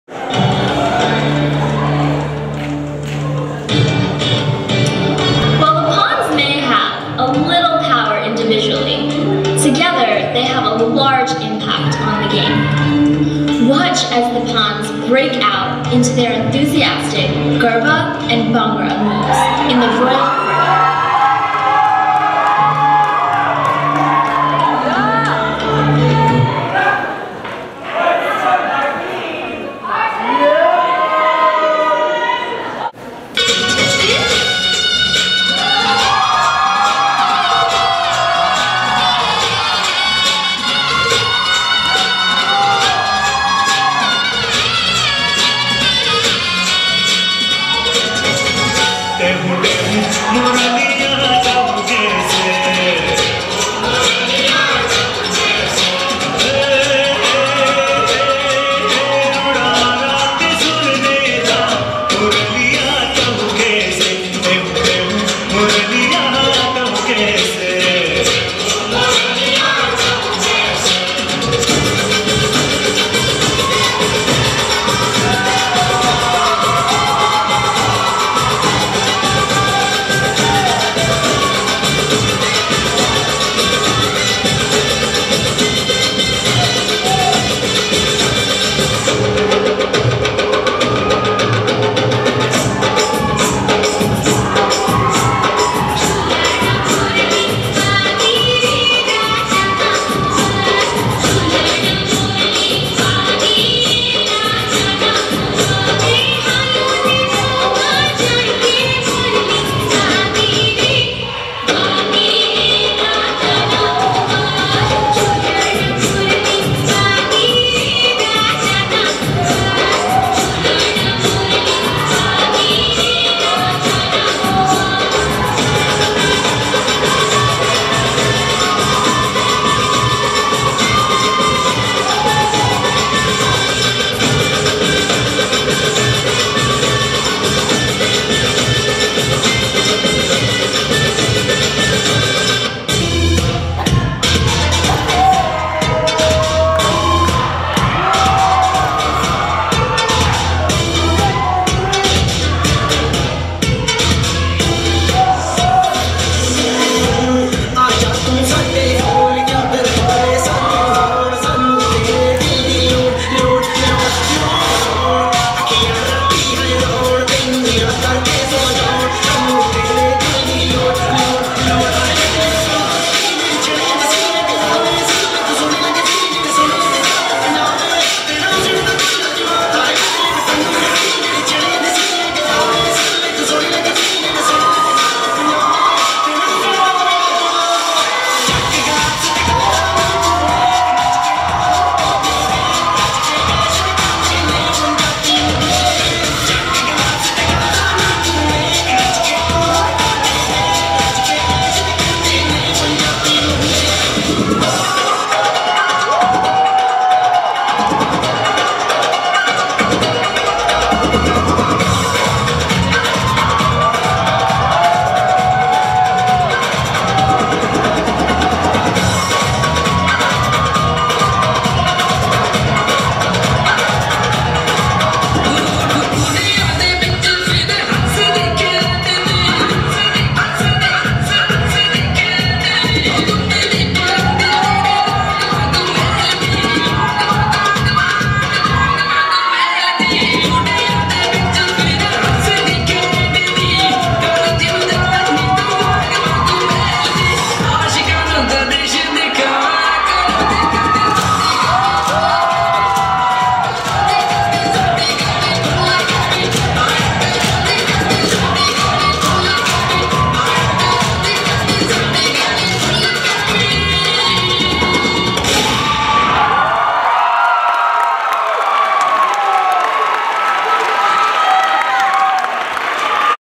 Garbhangra | All the Right Moves (PENNaach 2012)
Garbhangra Piece from PENNaach's 14th Annual Show called All the Right Moves in 2012